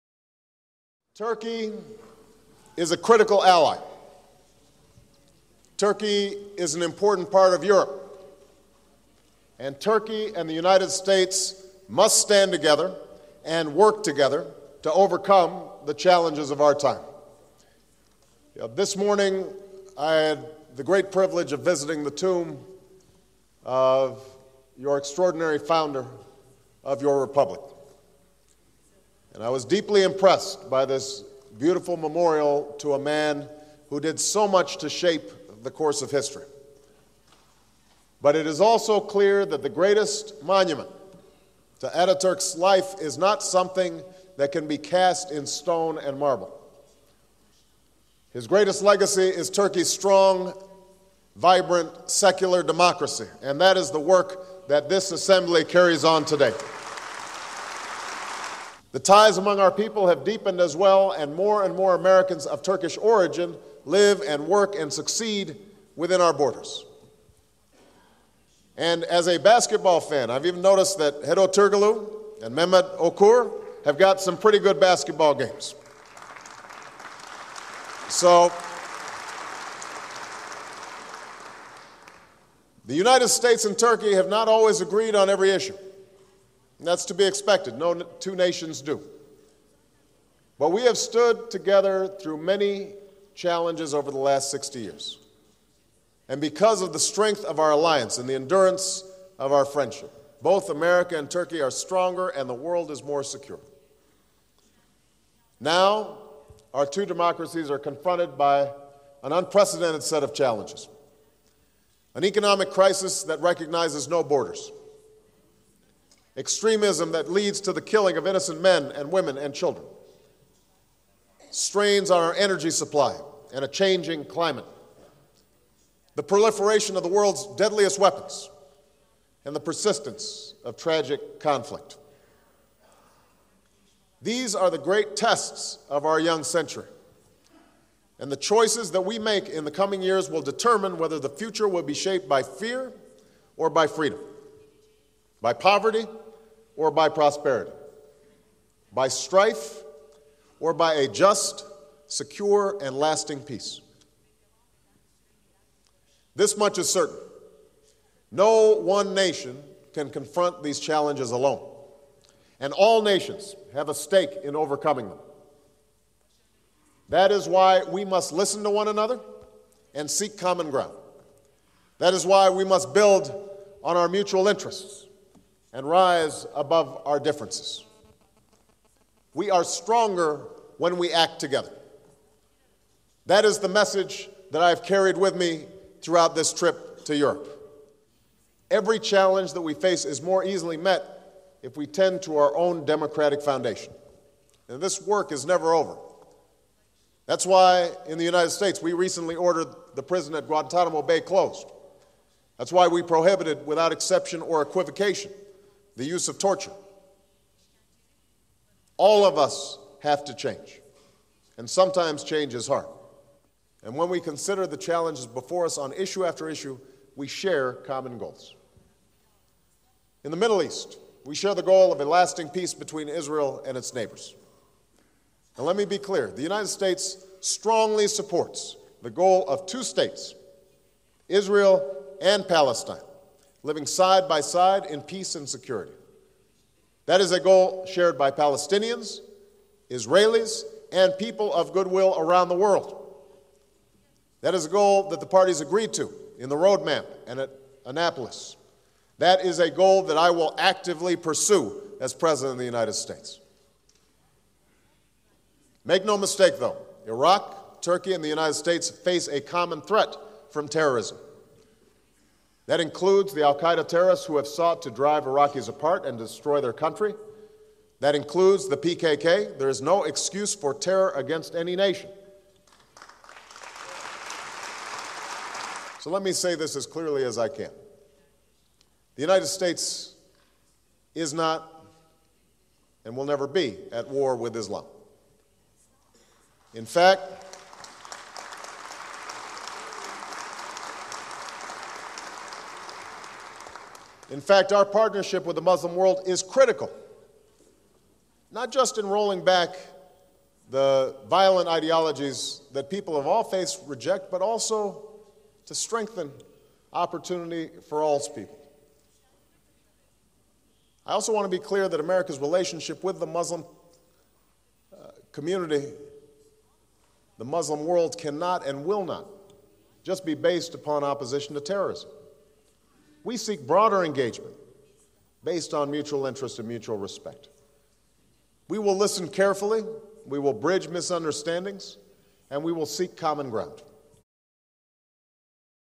Obama: US not at war with Islam
On Monday, the Associated Press reports, Obama, making his first visit to a Muslim nation as president, declared Monday the United States is not at war with Islam and called for a greater partnership with the Islamic world.

This video is from CNN.com, broadcast Apr. 6, 2009.
Video shared by Rawreplay